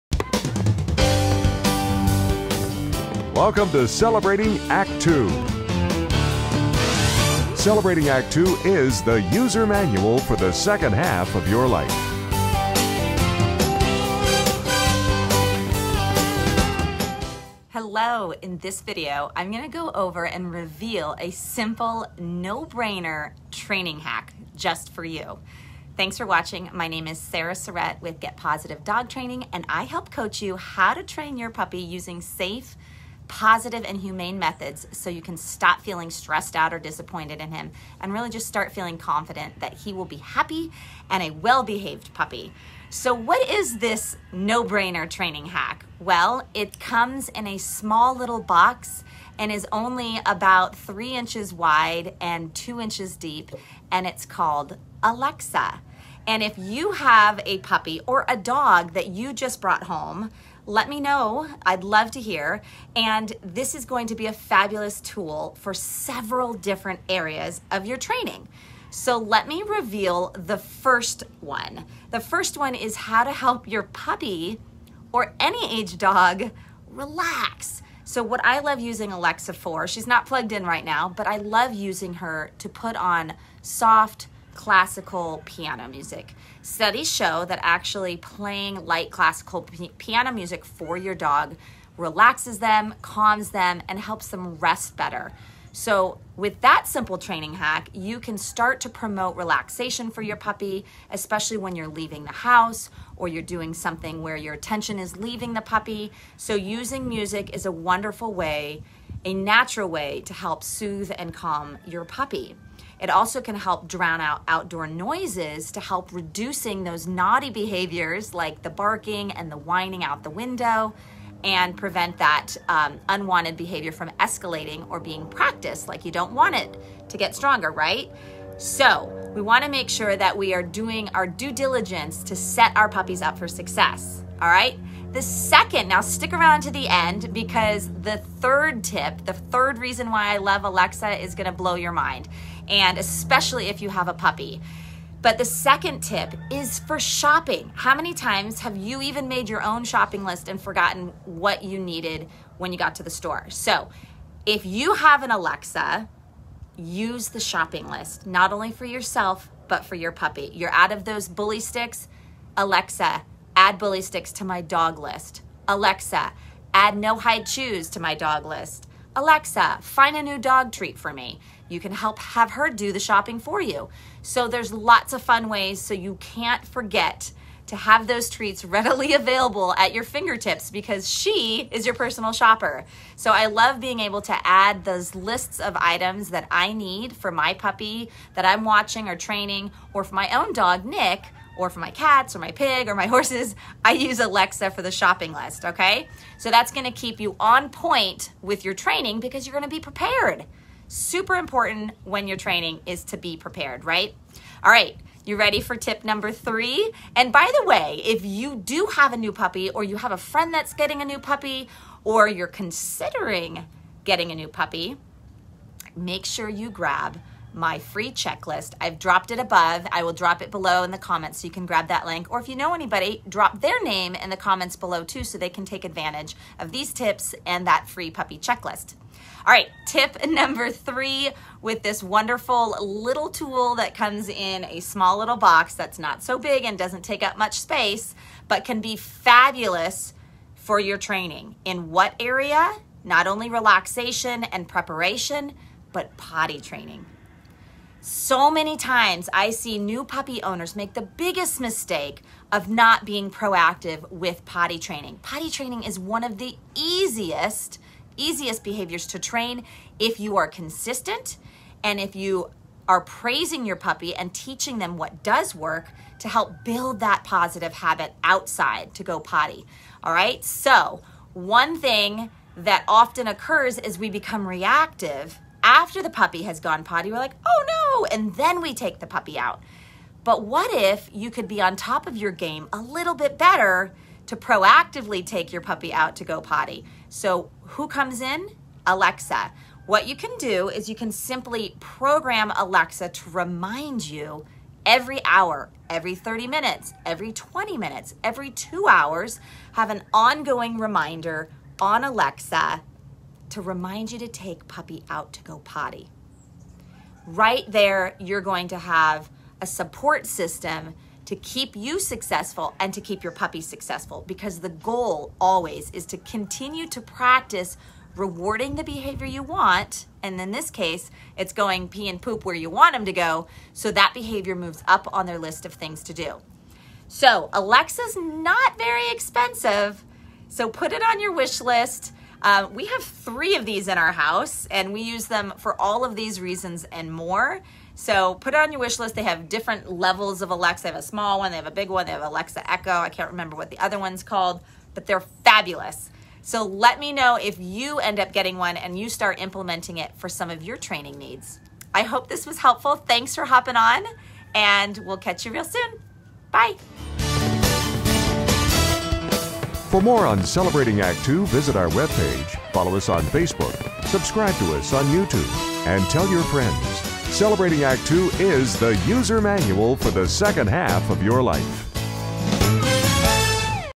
A Quick Dog Training Hack!
Animal Behavior and Training Expert, Sarah Surritt of Get Pawsitive, shares a quick tip for training your dog...it's a simple thing she says can help you 3 different ways! No matter your age or the age of your dog, you''ll find this useful!